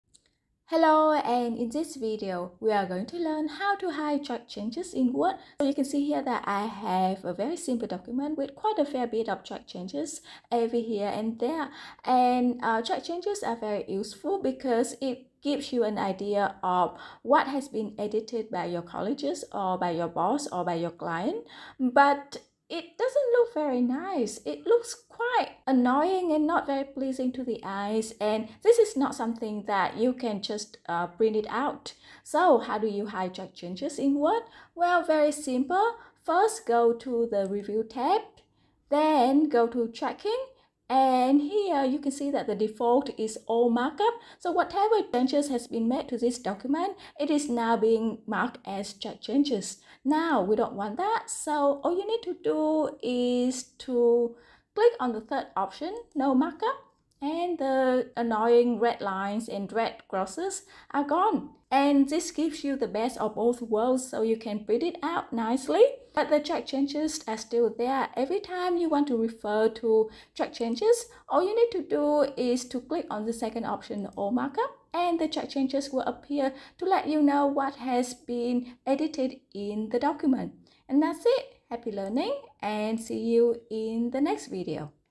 How to hide track changes in a Word document
Track changes are very useful in letting you know what have been edited by whom in the document.  But it doesn't look nice visually.  Let's learn how to have the best of both worlds - printing out a perfectly looking document while still keeping track of all track changes.  Let's learn how this can be done in this short, short video.  Let's go!!!